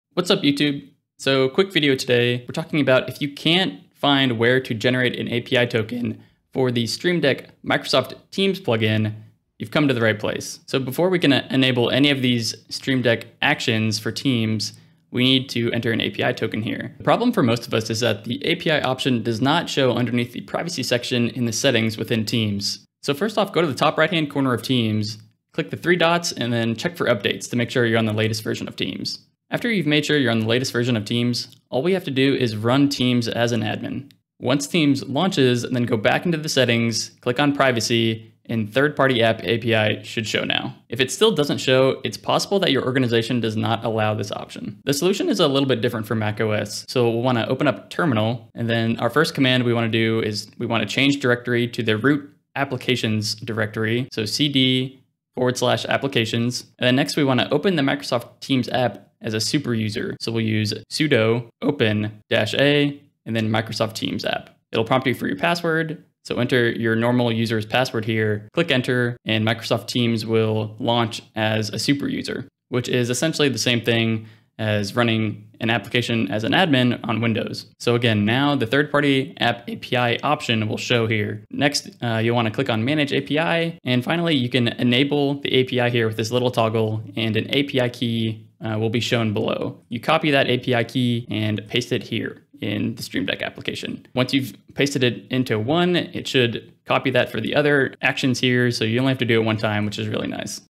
How to Enable Microsoft Teams API for Stream Deck
Update:

Looks like things have changed since I posted this video.

Original:

I'm not sure if this is the intention of Microsoft or if it's a bug, but in this video, I'll show you how to enable the Third-party app API option for both Windows and MacOS. test

The Microsoft Teams plugin for the Stream Deck just released (again)! I'm surprised how many different things I've used the Stream Deck for. This plugin would be very handy to have during calls to easily mute/unmute, turn on or off your camera, react, and leave the call.

Some of us are having trouble finding where to generate an API token. This is required, so if you are unable to generate an API token, the Teams plugin will not work.